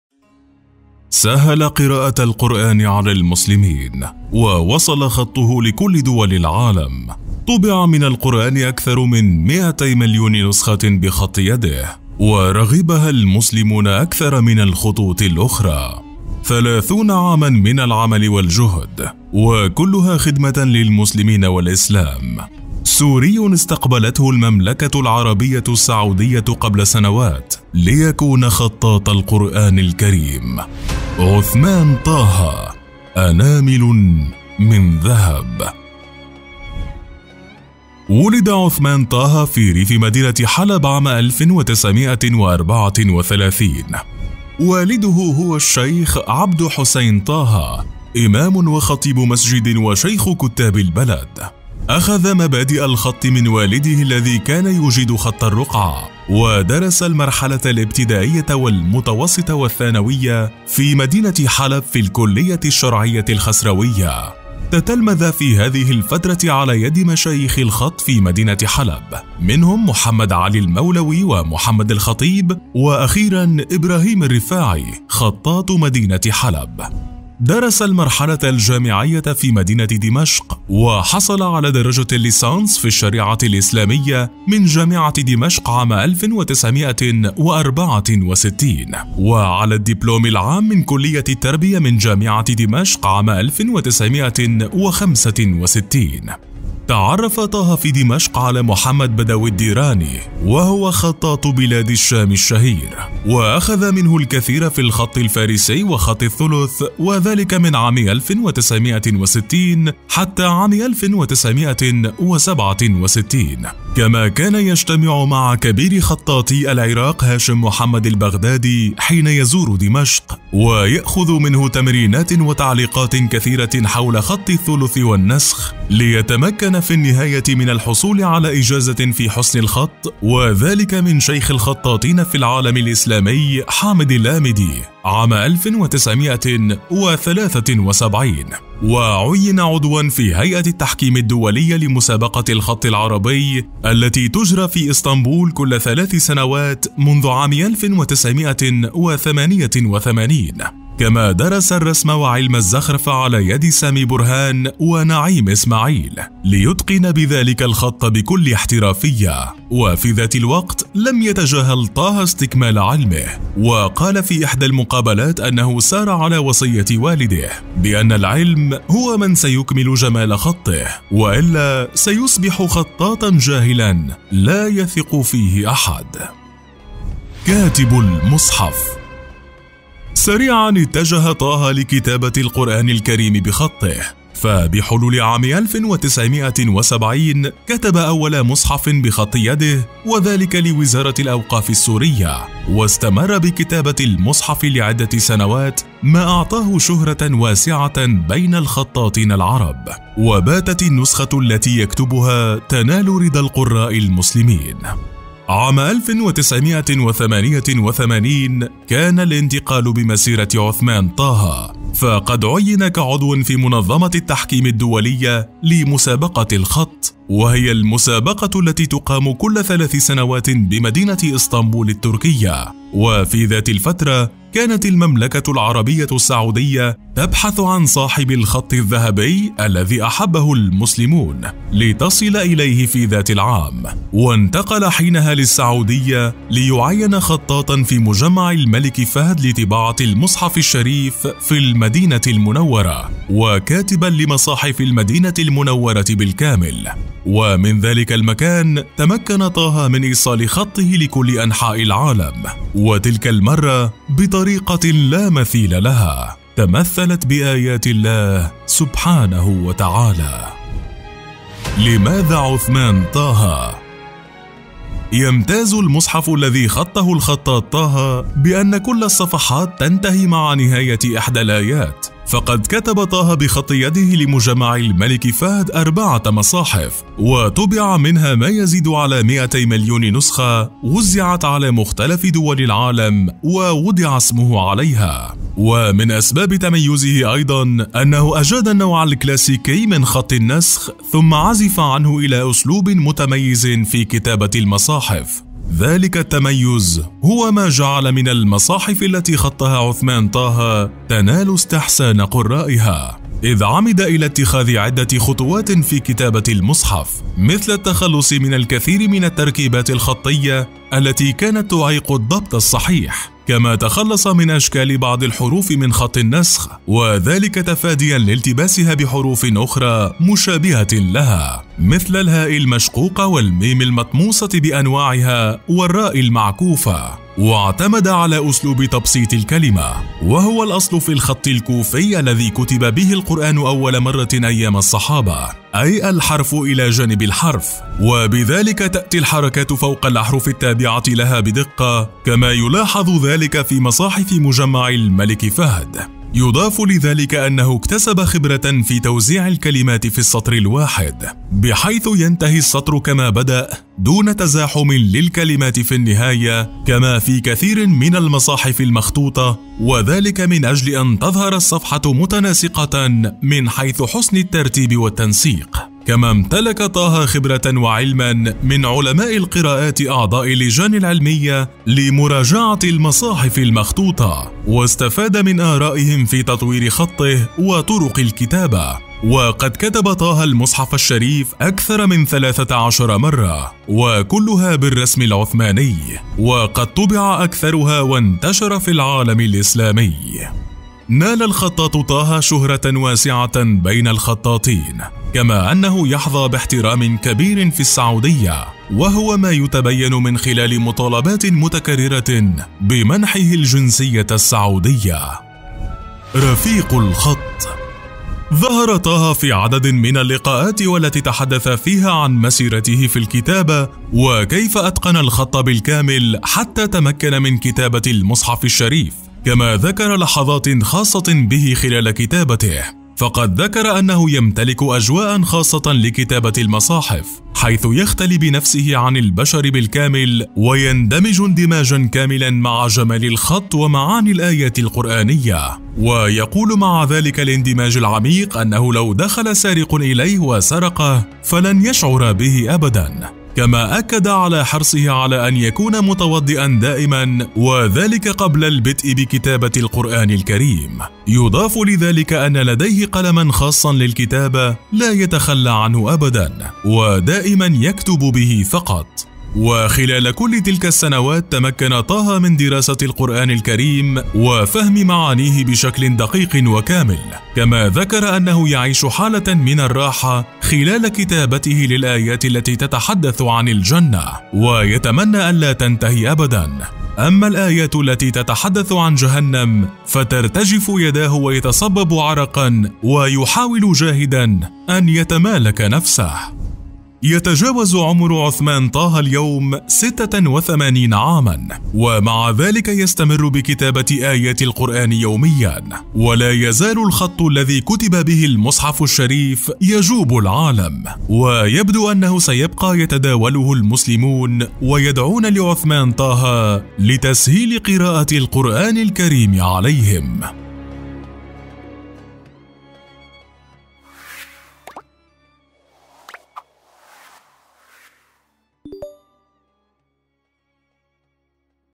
طُبع 200 مليون نسخة من القرآن الكريم بخط يده .. عثمان طه أناملً من ذهب
فيديو يروي قصة خطاط القرآن الكريم في مجمع الملك فهد لطباعة المصحف الشريف بالمدينة المنورة في المملكة العربية السعودية وكيف بدأ مشواره من مدينة حلب وحتى وصل للمملكة ليكون خطاط المصحف الشريف.